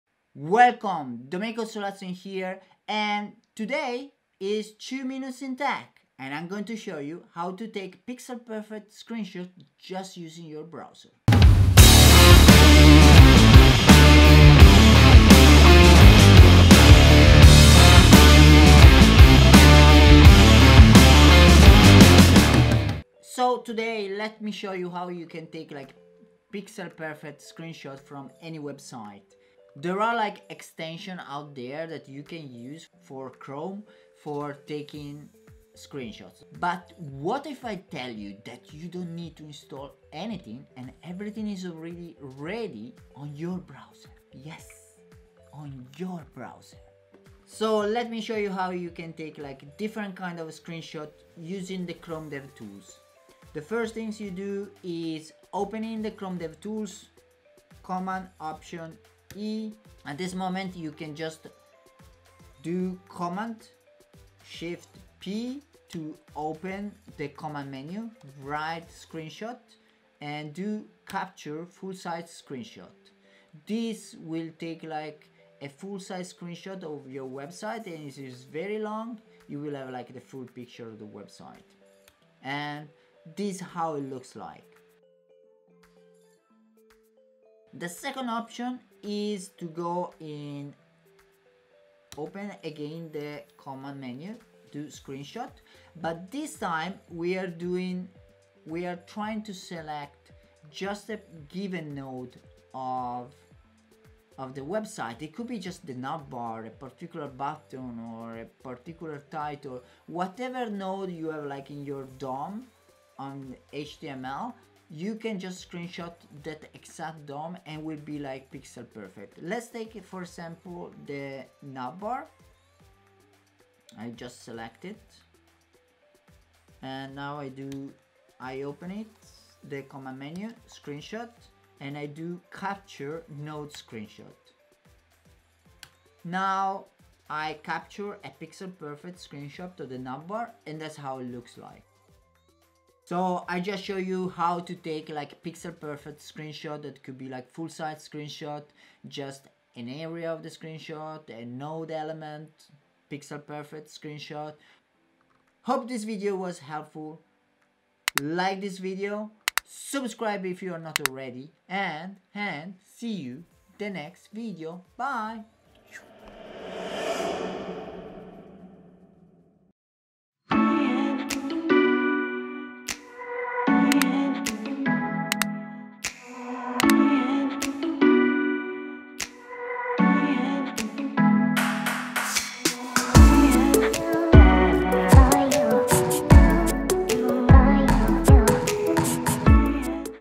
3 Must Know Chrome Dev Tools Tricks (Watch THIS!)
How many times have you tried to take a screenshot of your screen? Seriously..gazillions time!

In this video, I am going to show 3 must know tricks to take screenshots using the Chrome Dev Tools!

Super easy, super fast. No 3rd party extensions!

Let's GO!

What is your favorite trick using the Chrome DevTools?